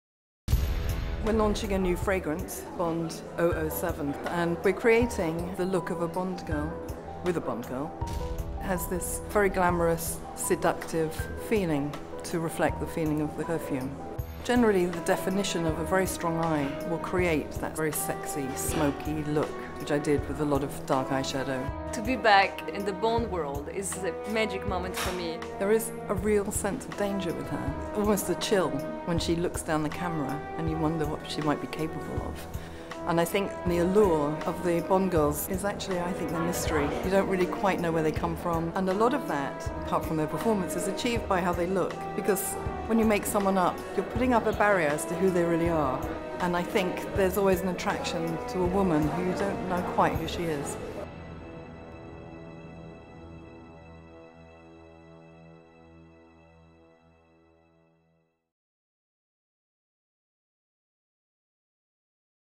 007 For Women Behind The Scenes – The Makeup
Watch the Behind the Scenes video exploring the makeup techniques used in the 007 for Women Fragrance TV ad.

Featuring CASINO ROYALE Bond actress Caterina Murino.